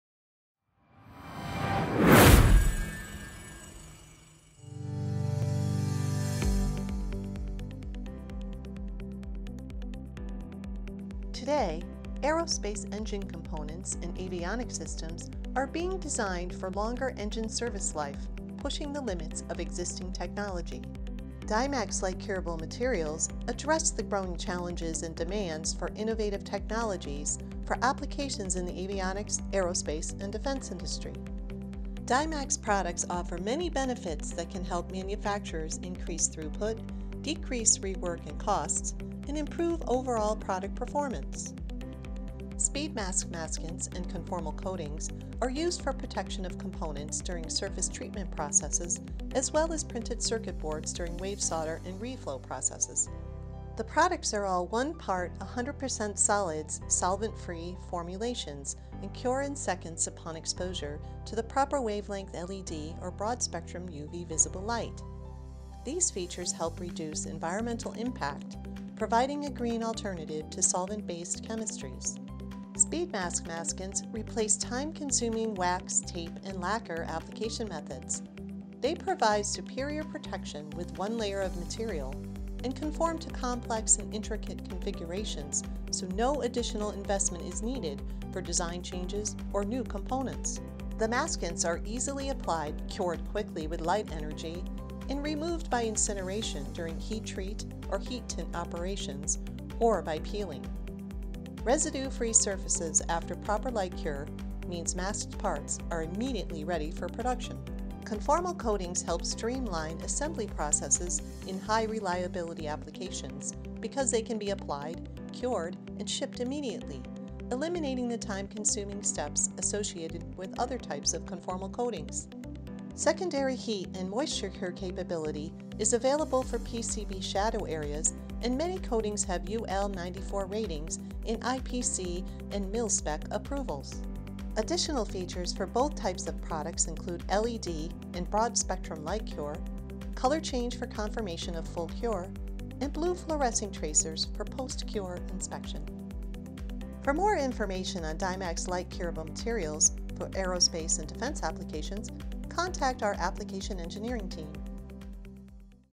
Dymax Video - Light-Curable Materials for Aerospace & Defense Applications
Dymax light-curable materials are specially designed to address the growing challenges of the aerospace and defense industries. Dymax speed mask maskants and conformal coatings provide a host of benefits compared to traditional methods, including increase throughput, decreased rework and costs, and improved overall product performance.

To learn more about Dymax light-curable materials and to shop online, visit our